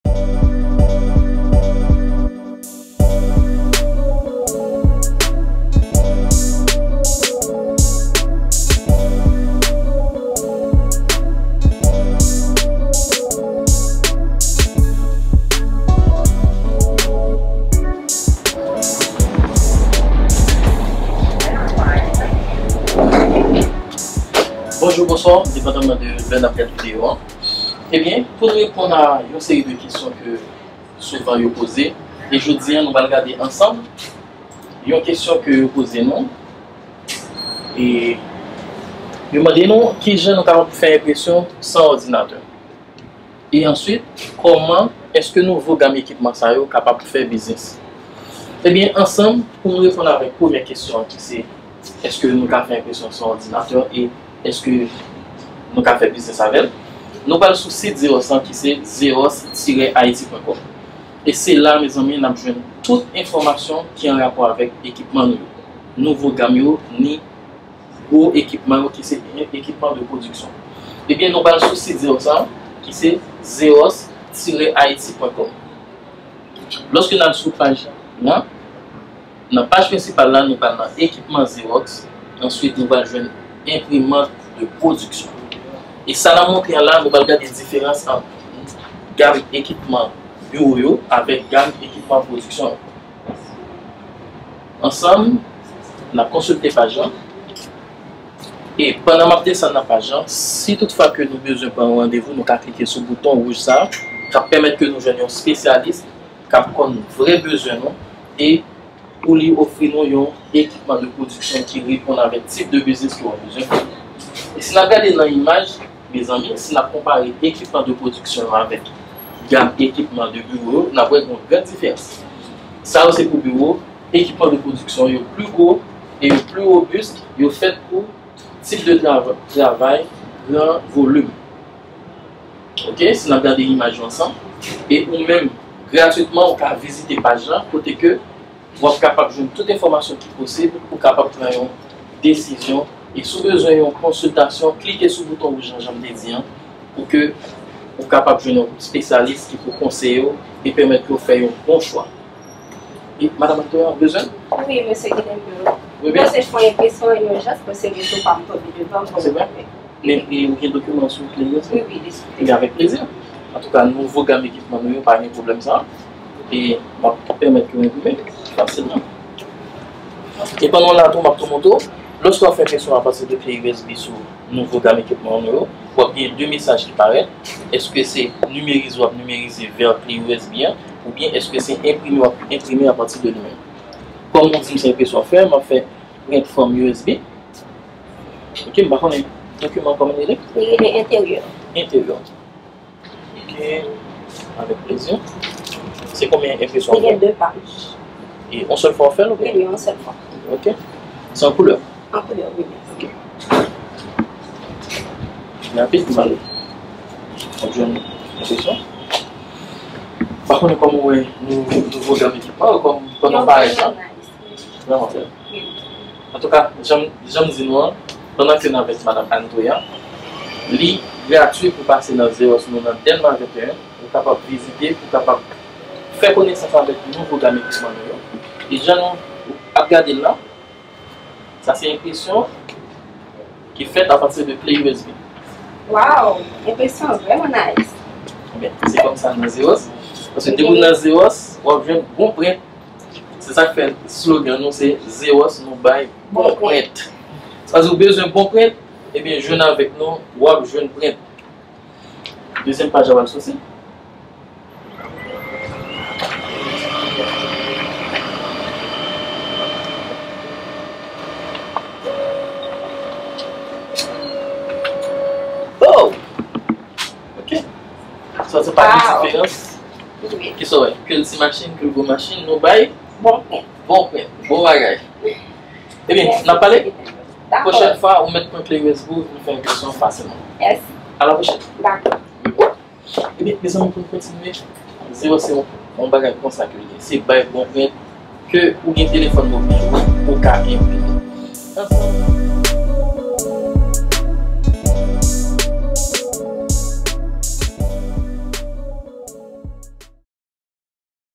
Comment imprimer si le réseau tombe ?
A travers cette vidéo, nous répondons à 2 questions qui ont été poser:

1 - Les équipements qui sont recommandés pour un service de copies ou impressions

2 - L'impression via USB-Direct ou USB-Print permet d'imprimer sans passer par un ordinateur ou un réseau informatique.